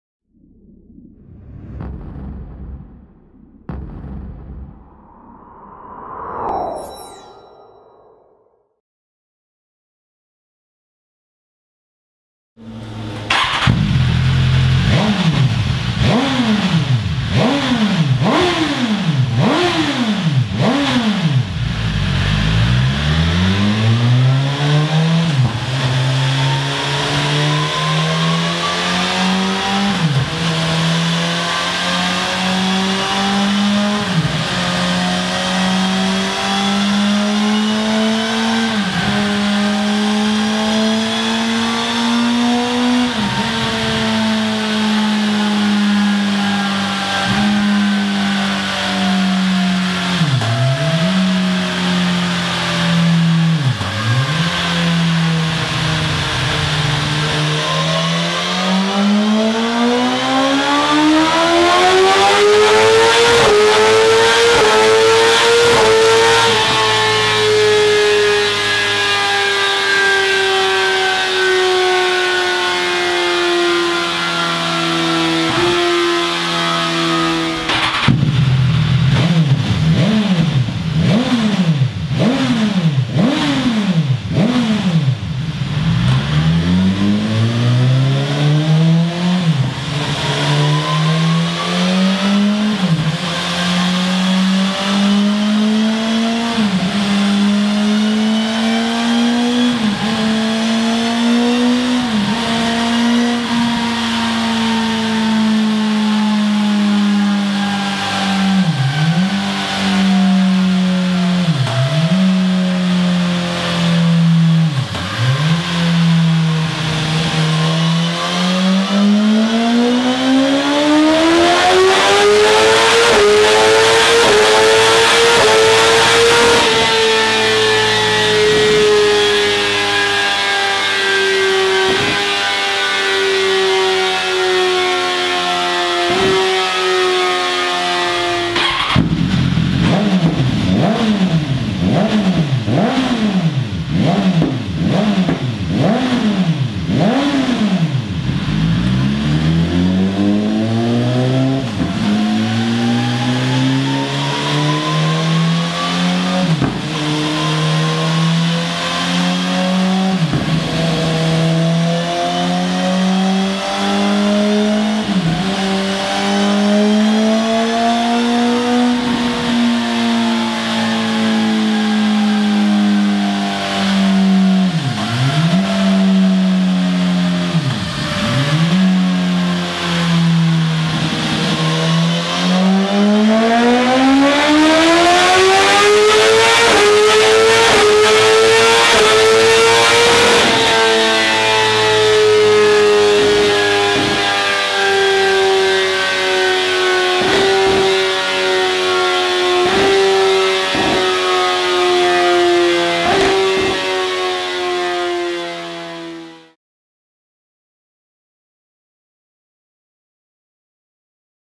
Scorpion Serket Taper and RP-1GP Performance Exhaust BMW S1000RR
Video from Scorpion Exhausts showcasing the change in performance and sound when the BMW S1000RR is upgraded with a Serket Taper or a RP-1GP exhaust.

Moto Machines is the U.S. importers of Scorpion Exhausts and have all the different exhausts available for many different motorcycle models!

Moto Machines

45891 Woodland Rd. #115